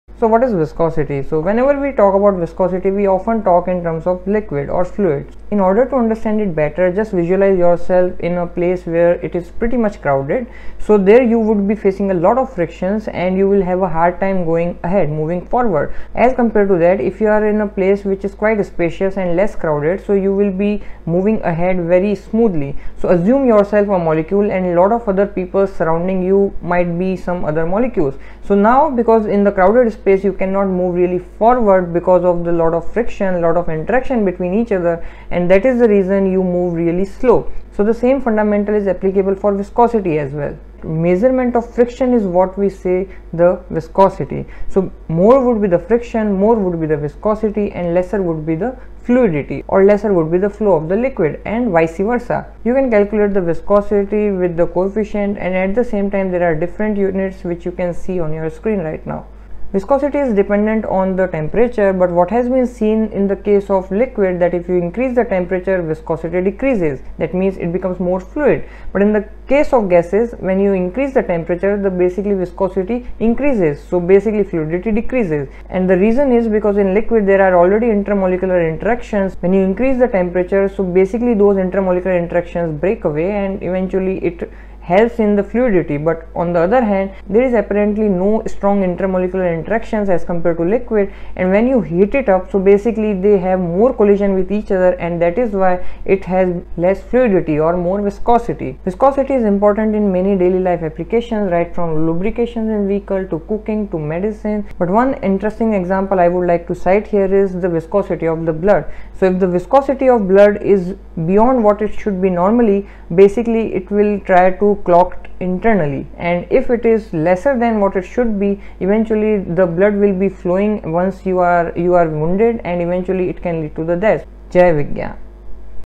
What is Viscosity | Dr. Priyank Singhvi
Viscosity describes the internal friction of a moving fluid. A fluid with large viscosity resists motion because its molecular makeup gives it a lot of internal friction. A fluid with low viscosity flows easily because its molecular makeup results in very little friction when it is in motion.

Produced and researched by Dr. Priyank Singhvi